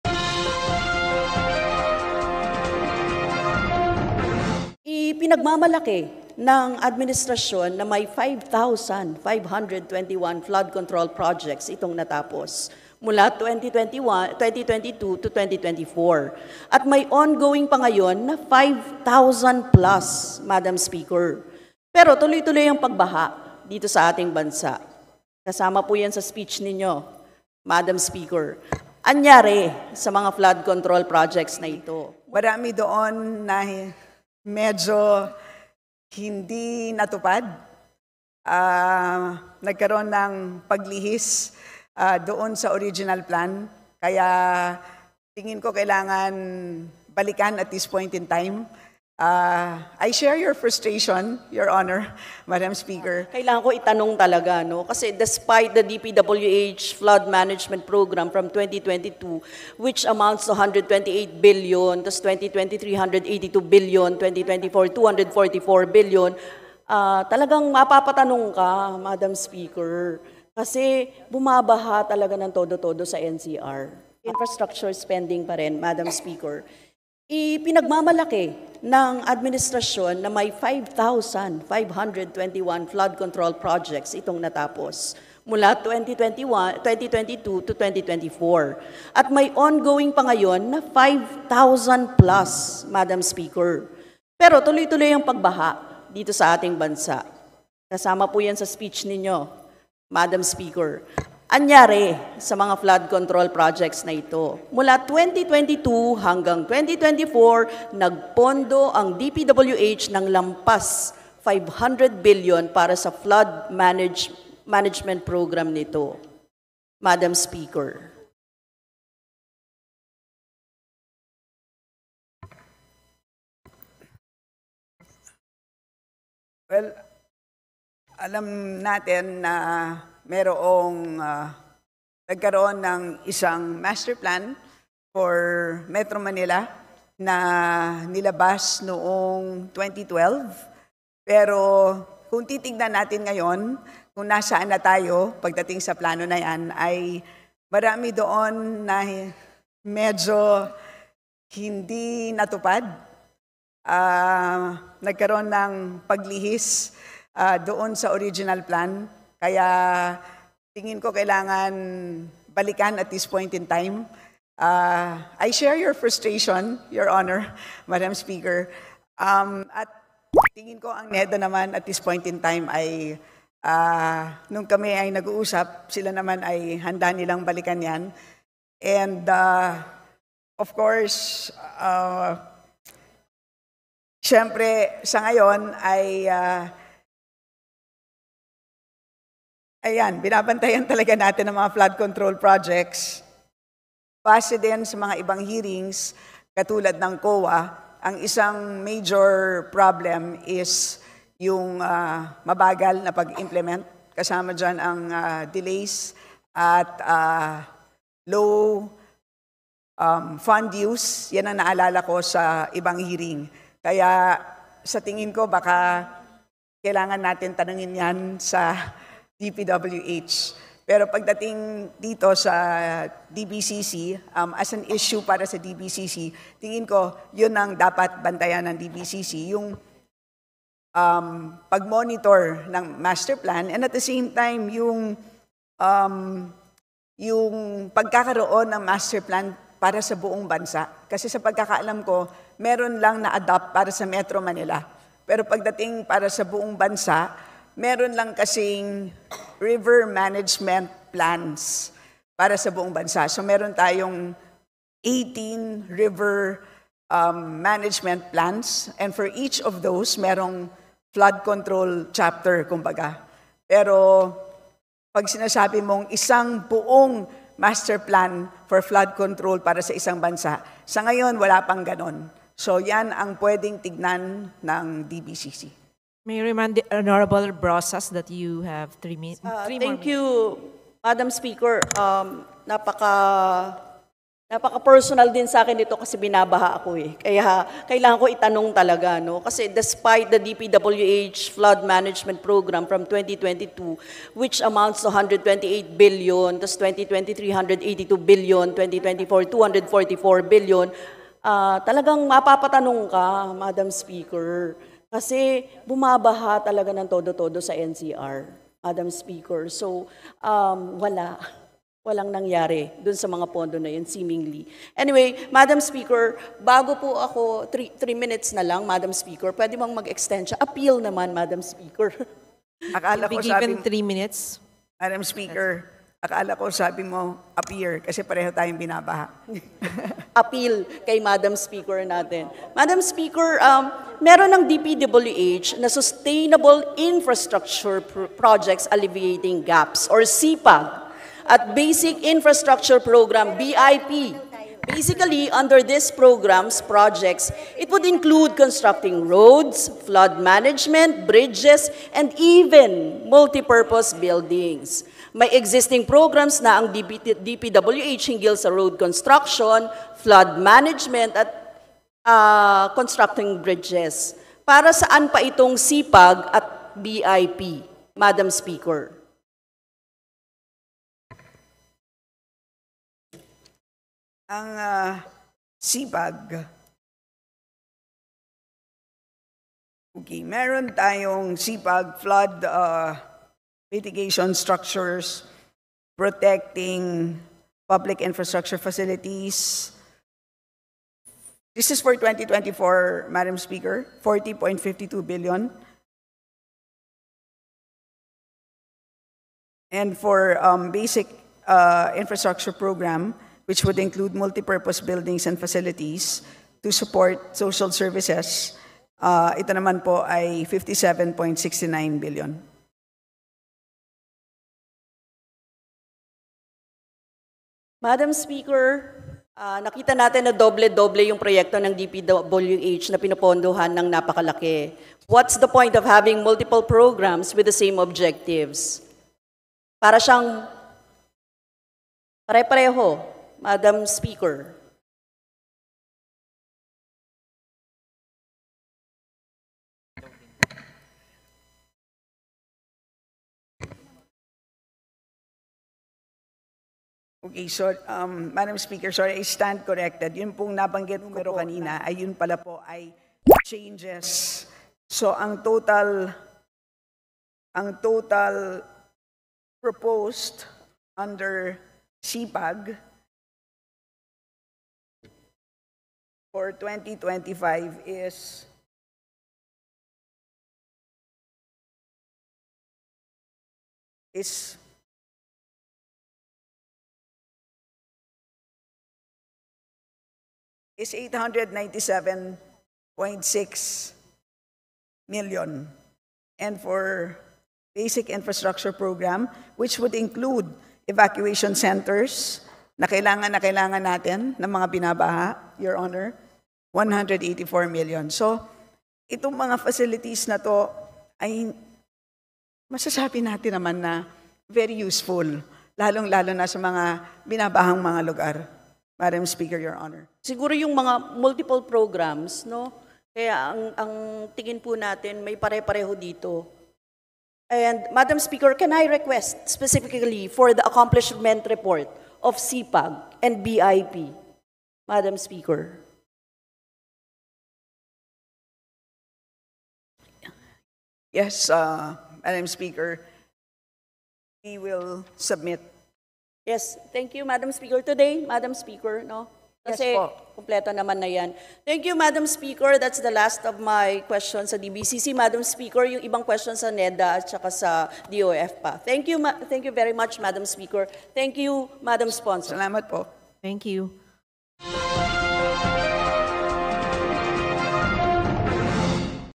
Cong Quimbo: NAPAHIYA, PALPAK NA PAGGASTOS SA FLOOD CONTROL PROJECT NABISTO
REP. STELLA QUIMBO, UMAMIN NA MARAMI ANG HINDI NATUPAD
SA FLOOD CONTROL PROJECTS